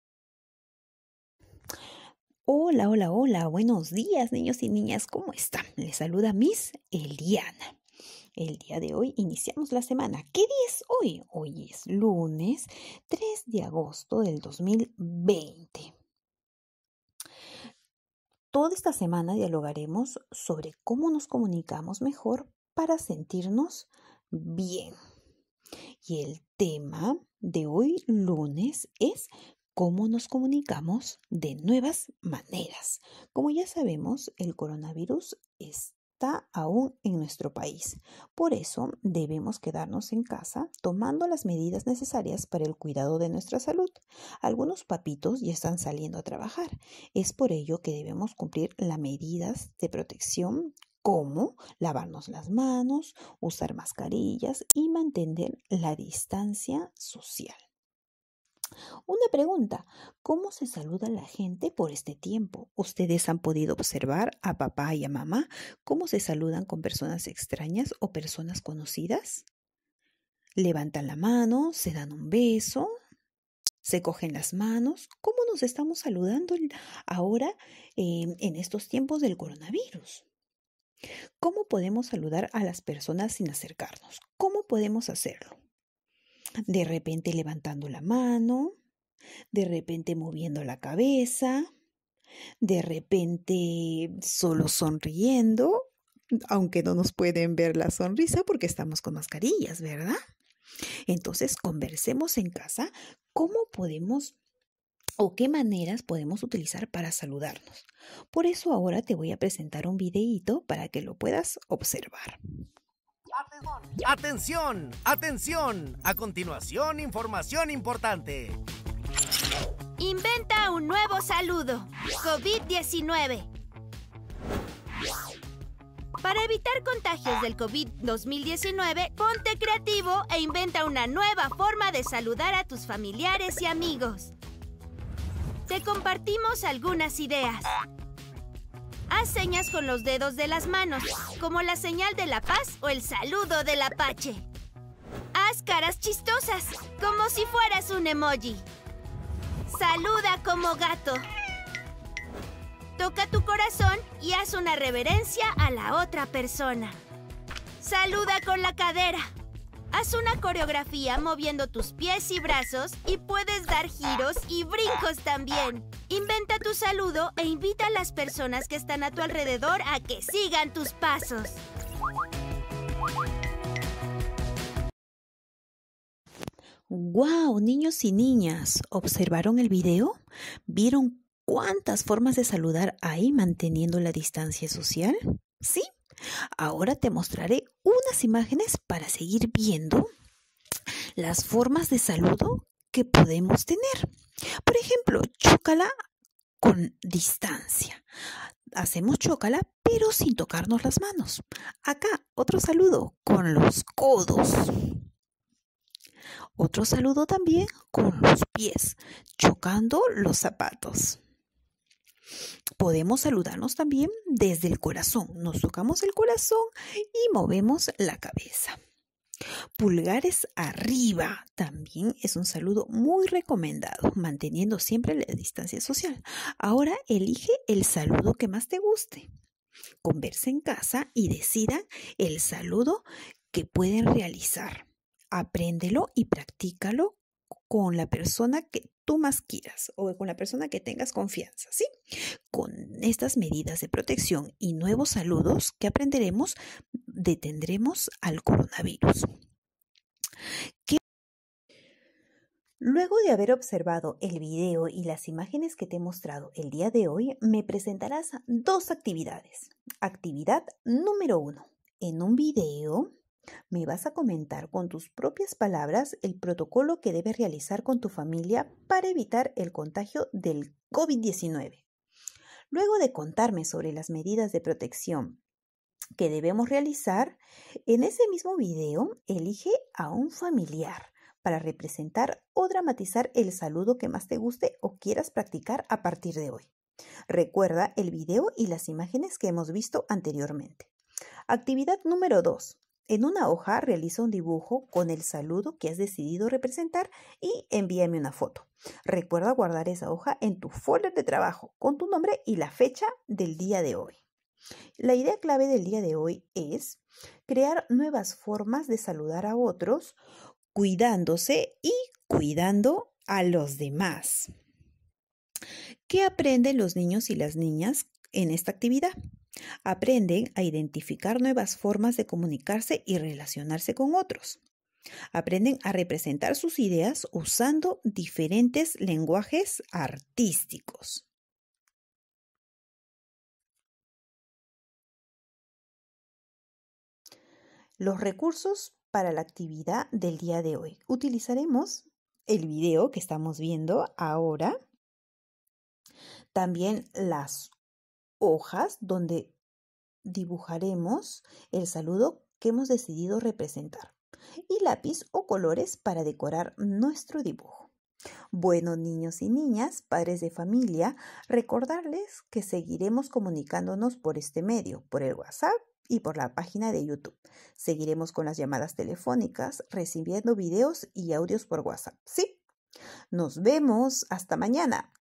Nos comunicamos de nuevas maneras 🤗- Aprendo en casa 📺- Miss Eliana 03-08-2020
Propósito de la semana:

Que las niñas y los niños tengan la oportunidad de reconocer, explorar y expresar sus emociones en un contexto  en  el  que  muchas  formas  de  relacionarnos  han  cambiado  debido  a  la  emergencia  sanitaria.  Estas nuevas situaciones generan emociones que pueden producir conflictos que necesitan ser resueltos con el acompañamiento de los adultos, escuchando y ayudando a poner en palabras las emociones e ideas de las niñas y los niños, para establecer acuerdos que favorezcan el bien común.